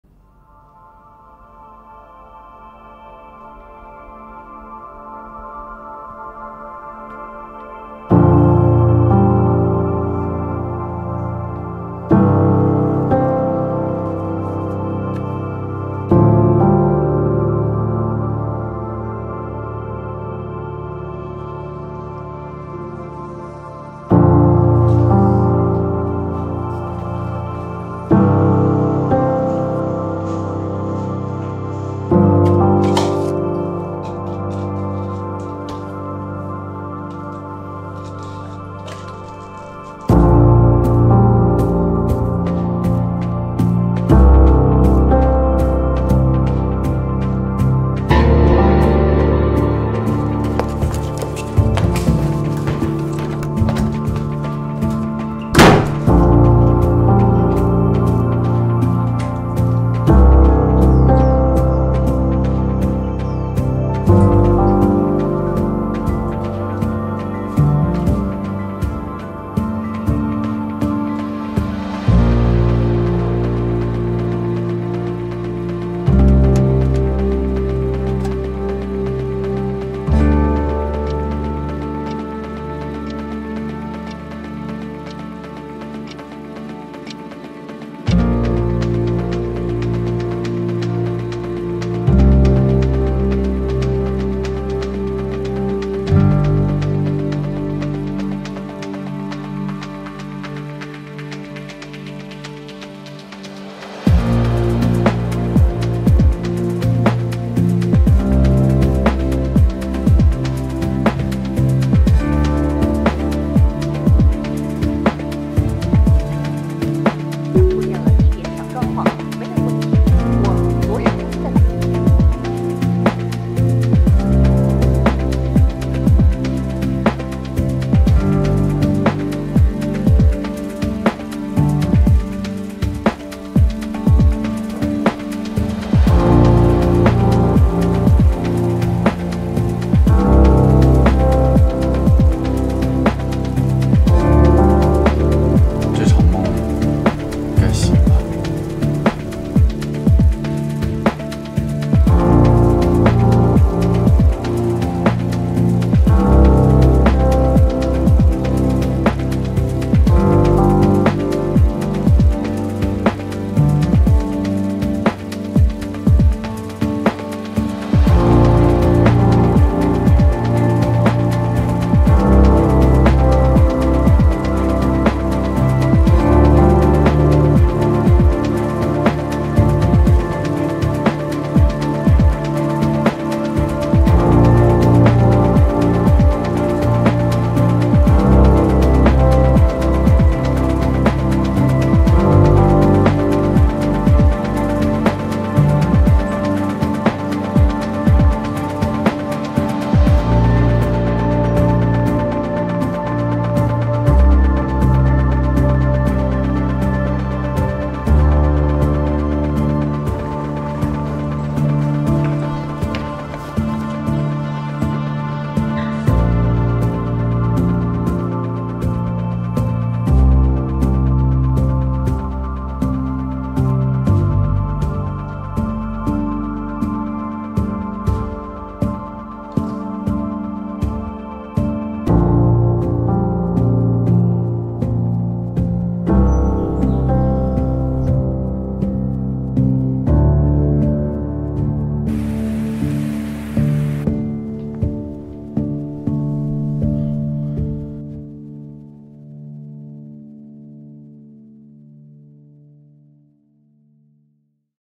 Filmmaking Showreel 2020-2023 - Wang Xin
This showreel contains drama short films I shot in college
Works as director:
"Panama"
"Tidewater"
"Summer insects"
"My sister"
"The land passes into silence"

Works as Cinematographer:
"Beans II"
"Loading phantom"

Looking back at the short films of the past few years, the scene during the filming is still vivid in my mind.
Thanks to everyone who worked hard to make them possible.